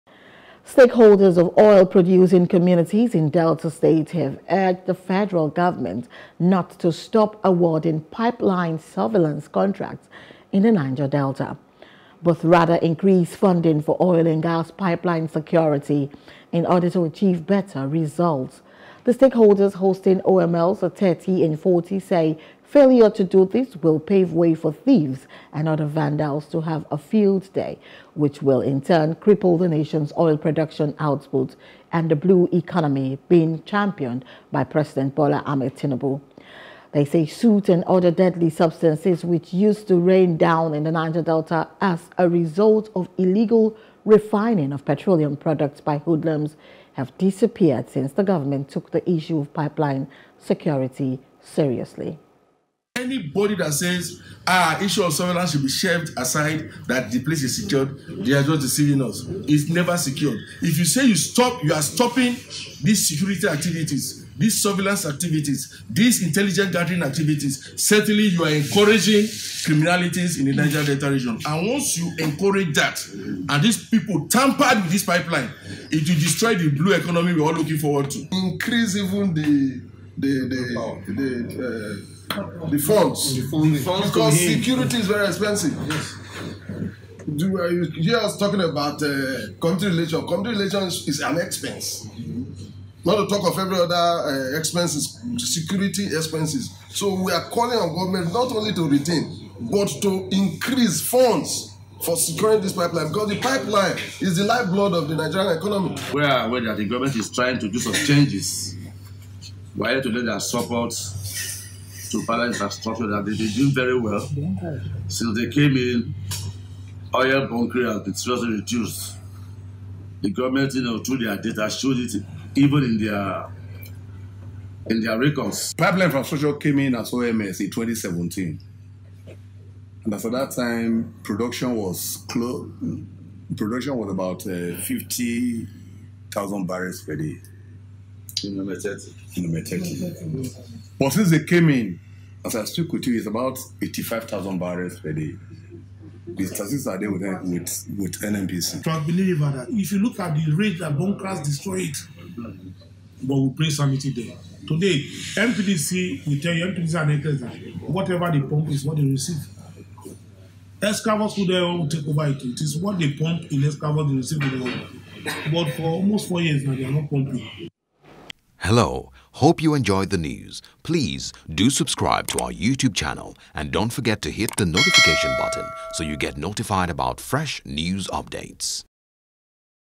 Stakeholders Urge Federal Givernment To Increase Funding For Pipeline Security
Stakeholders of oil producing communities in Delta State have urged the federal government not to stop awarding pipeline surveillance contracts in the Niger Delta; but rather increase funding for oil and gas pipeline security - in order to achieve better results.